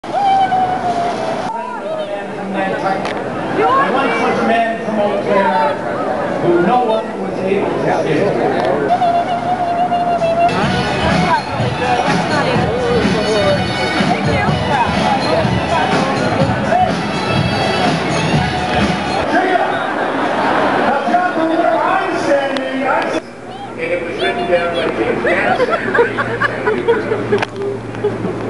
Beaker visits the rally.
This is a Halloween costume I made for my partner. Then we went to the Seattle Rally to Restore Sanity. It was fun.